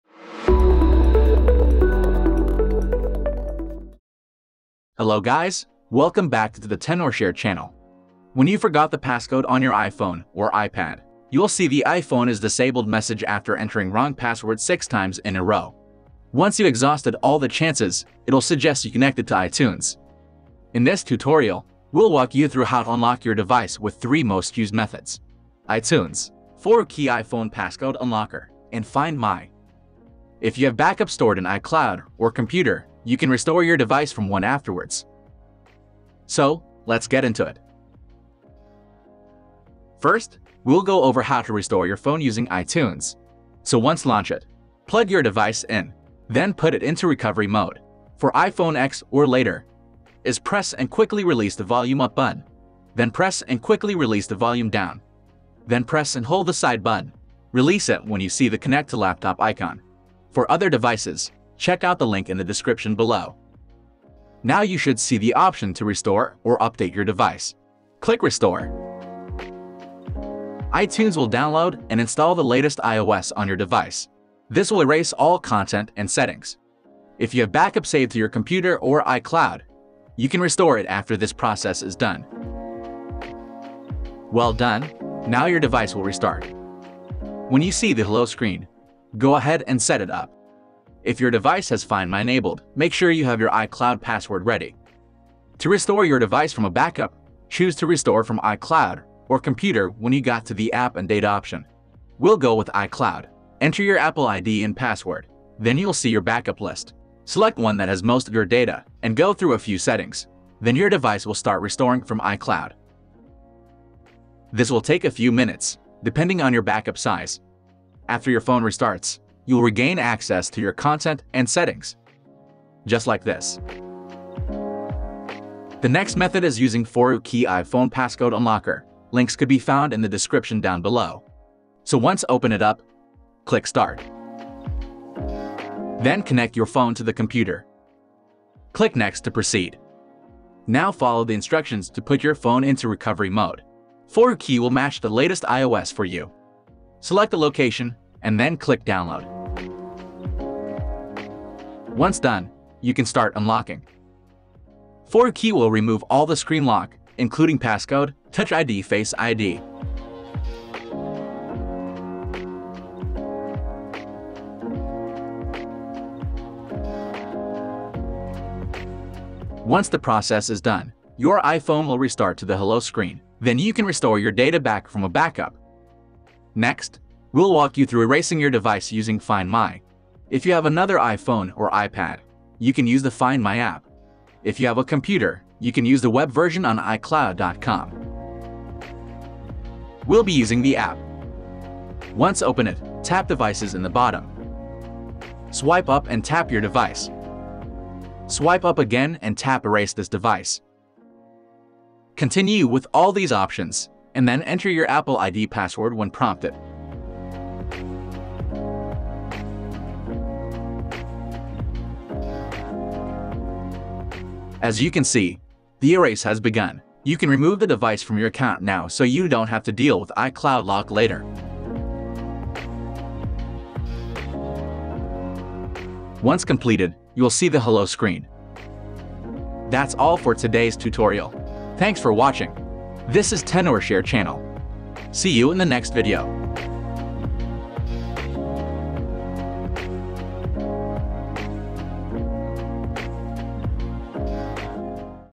iPhone is Disabled Connect to iTunes? 3 Ways to Fix it! | 100% Work | 2024
If you forgot your iPhone passcode and it's showing iPhone is disabled connect to iTunes message, learn how to unlock it with 3 methods in this tutorial.

Chapters:
0:00 Intro & Preview
0:43 Method 1 - Restore iPhone using iTunes
1:30 Restore iPhone from iCloud Backup
2:11 Method 2 - Unlock iPhone with 4uKey (No iTunes)
3:01 Method 3 - Erase iPhone in FInd My (No Computer)

1. Method 1 - Restore iPhone using iTunes:
Launch iTunes on your computer.
Plug in your device and put it into recovery mode.
Choose the "Restore" option in iTunes to download and install the latest iOS version, erasing all content and settings.
After the process completes, set up your device and restore from a backup if available.

2. Method 2 - Unlock iPhone with 4uKey (No iTunes):
Open 4uKey on your computer and click "Start."
Connect your device and follow the instructions to put it into recovery mode.
Download and select the iOS version in 4uKey.
Begin the unlocking process, which removes all screen locks.
After completion, set up your device and restore data from a backup.

3. Method 3 - Erase iPhone in FInd My (No Computer):
Use the Find My app on another iOS device or visit iCloud.com on a computer.
Select your locked device and choose the "Erase This Device" option.
Confirm the erase and enter your Apple ID password.
Once completed, set up your device as new.
These steps offer different approaches to unlock a disabled iPhone or iPad, providing options for users based on their preferences and available resources.

⚠Important notice:
2. 4uKey will erase all content and settings from the device just like iTunes.
3. You can restore your device from a backup after the unlocking if you have backups stored in iCloud or your computer.
4. 4uKey is unable to obtain access to credentials, compromise personal data or cause serious harm to others. Do not try to violate YouTube community guidelines.